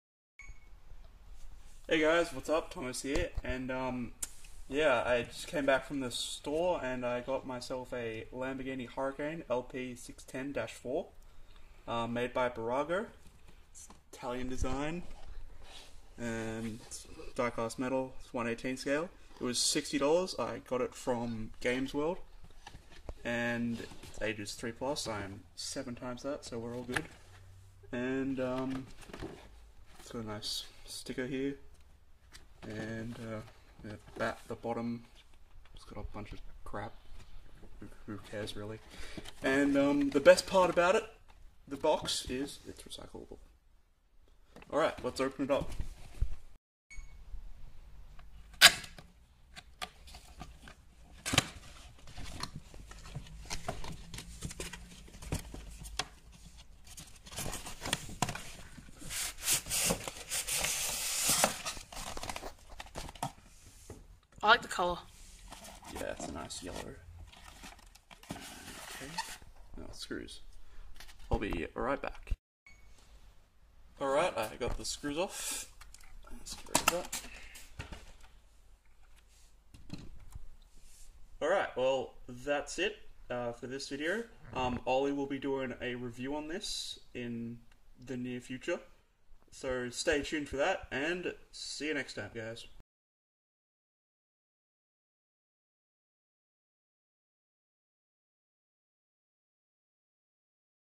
Tosttr Gaming Unbox a Lamborghini Huracan lp 610-4 by bburago
Thomas gets an awesome model from games world and shows it off to you guys